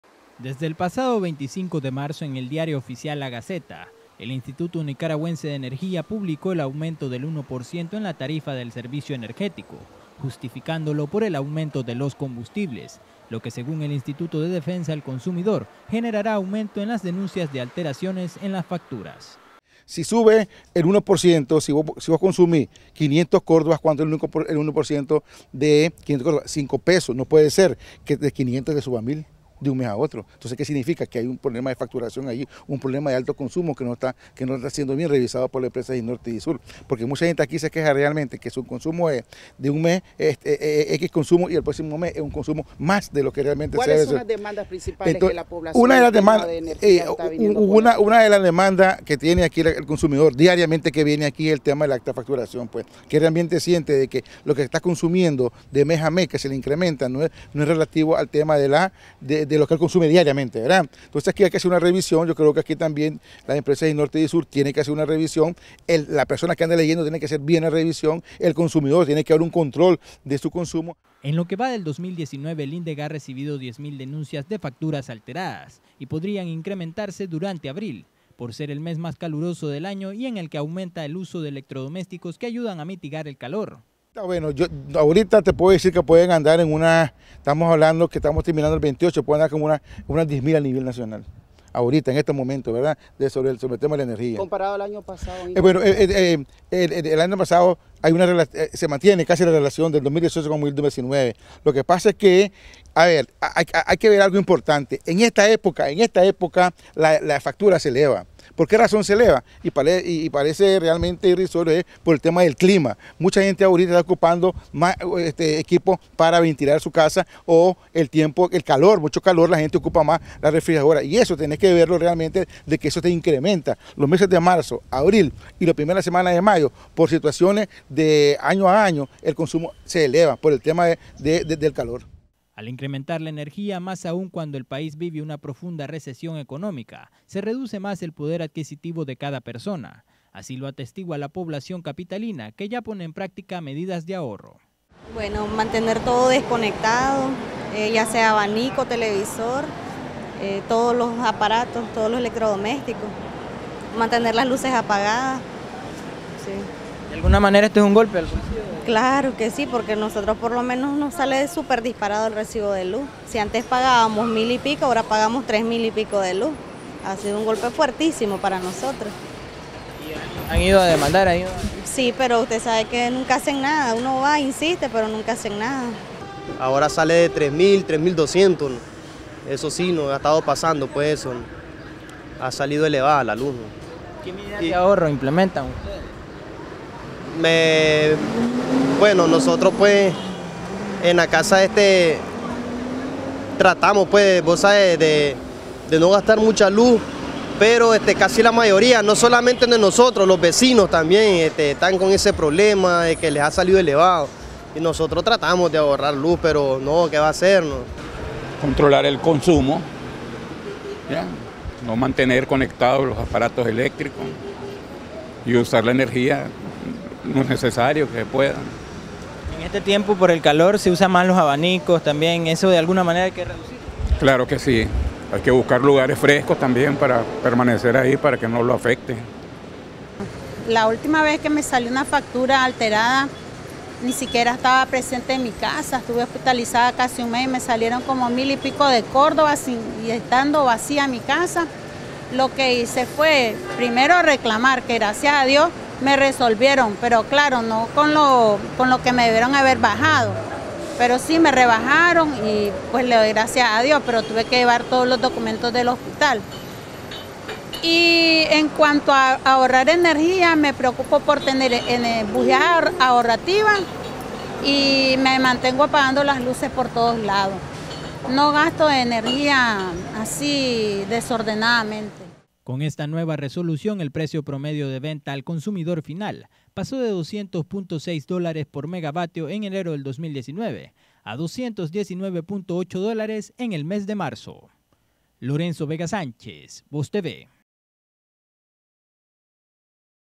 INE sube 1% a la tarifa de luz eléctrica
De manera sorpresiva apareció una publicación en el Diario Oficial La Gaceta, donde el Instituto Nicaragüense de Energía (INE) anunció el aumento del 1% en la tarifa de la luz eléctrica, justificándolo por el aumento de los combustibles que desde enero de este año ha subido hasta 23.63 córdobas.